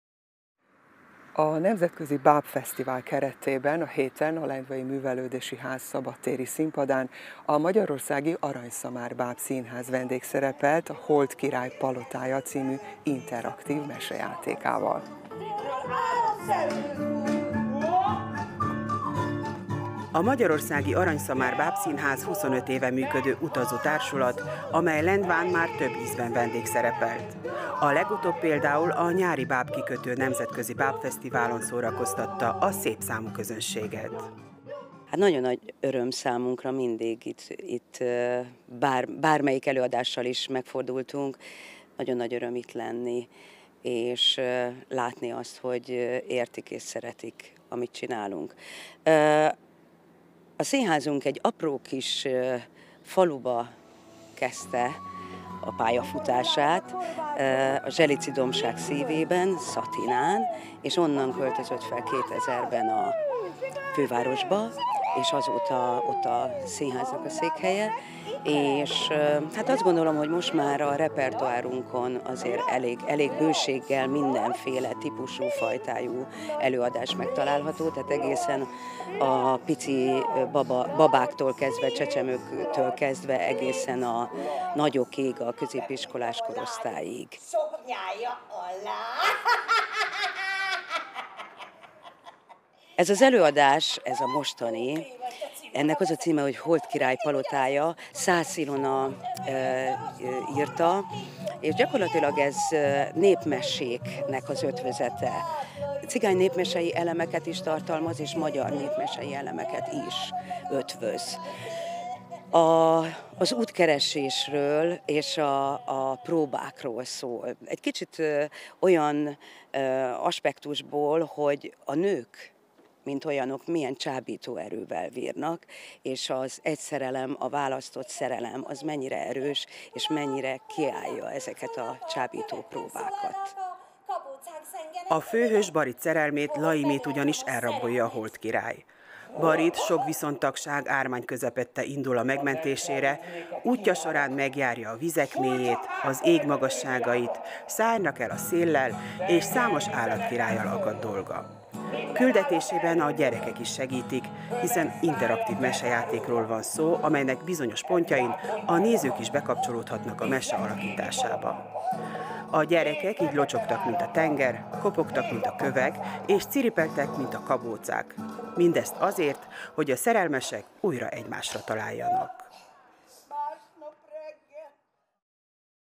Aranyszamár Színház: Lendván járt a Holdkirály Palotája című előadás
Nemzetközi bábfesztivál keretében Lendván járt az Aranyszamár Színház a Holdkirály Palotája című előadásával.